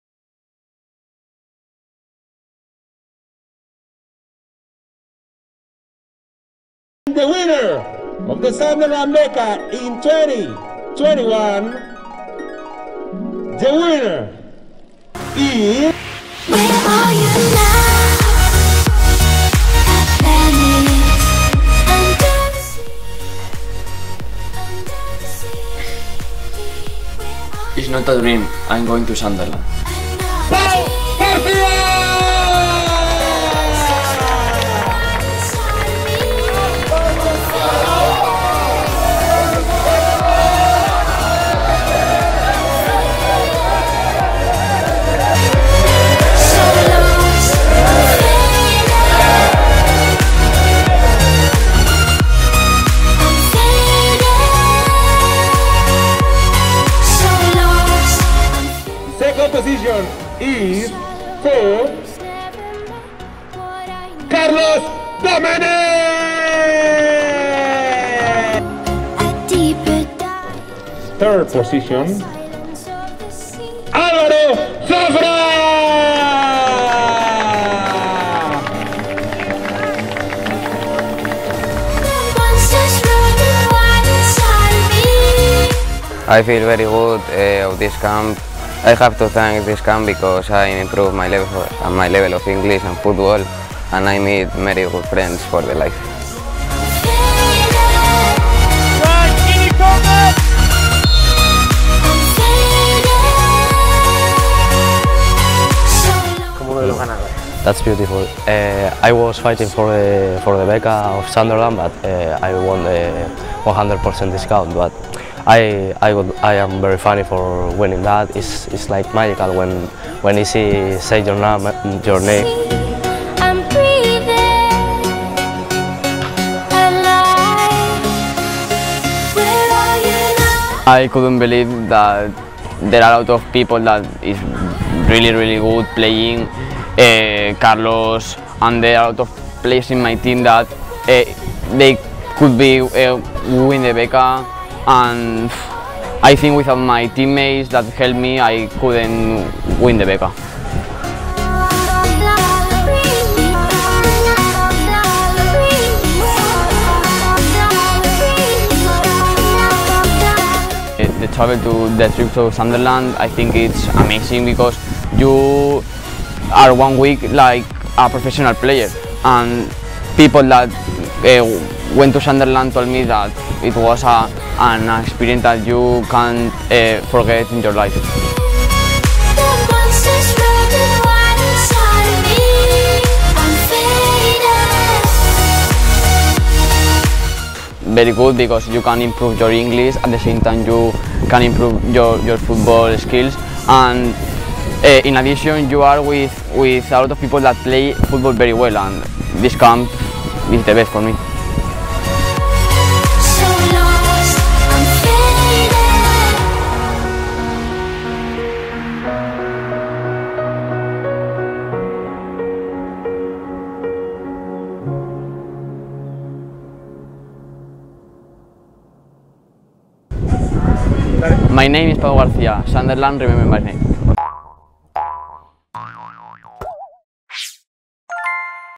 CAMPUS SUNDERLAND AFC LA NUCIA - DÍA 14
Últimos instantes de fútbol en la XI edición del Campus del Sunderland La Nucía 2021.
Como es costumbre, nuestros jóvenes futbolistas nos han hecho vibrar con un torneo de exhibición mientras sus familiares iban ocupando las gradas de la Ciudad Deportiva Camilo Cano.
Papás y mamás han podido conocer de primera mano cómo trabaja el cuerpo técnico del Campus, desde coordinadores, entrenadores, monitores, staff, enfermería y equipo de comunicación.
Ahora sí, ha llegado el momento... tenemos el enorme placer de desvelar los nombres de los ganadores de las 3 Becas que otorga el Campus del Sunderland AFC La Nucía en su segunda semana! Enhorabuena de todo corazón, campeones! Habéis sido la combinación perfecta de compañerismo, fair play, valores, voluntad por el aprendizaje del idioma y esfuerzo por mejorar futbolísticamente. Vosotros sois Inglaterra en Casa!
Enjoy! Nos vemos el año que viene!